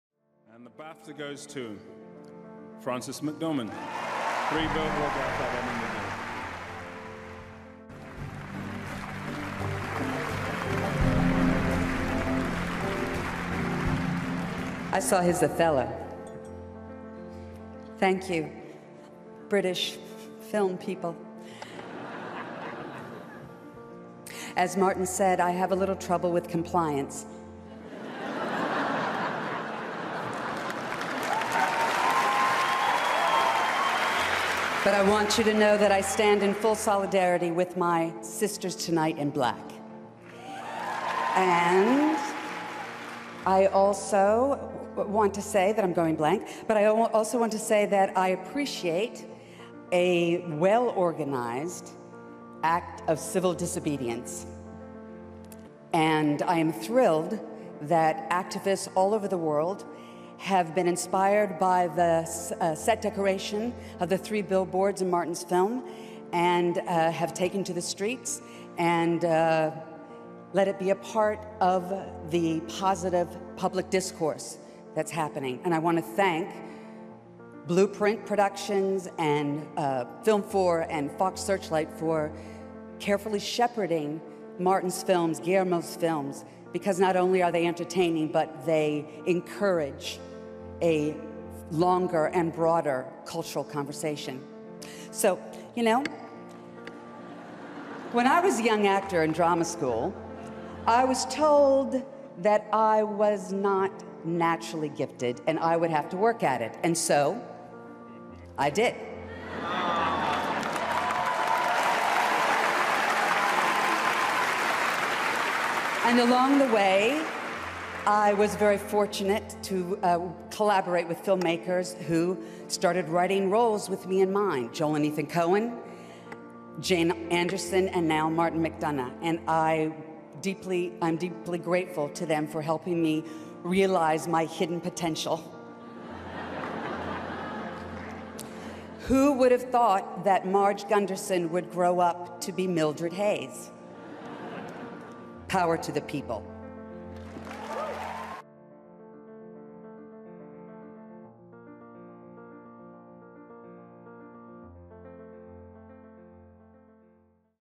Frances McDormand Speech at 71th British Academy Film Awards 2018 BAFTA By Hollywood Clips
The 71st British Academy Film Awards, more commonly known as the BAFTAs, were held on 18 February 2018 at the Royal Albert Hall in London, to honour the best national and foreign films of 2017

Discurso de Frances McDormand en la 71ª edición de la British Academy Film Awards 2018 BAFTA de Hollywood Clips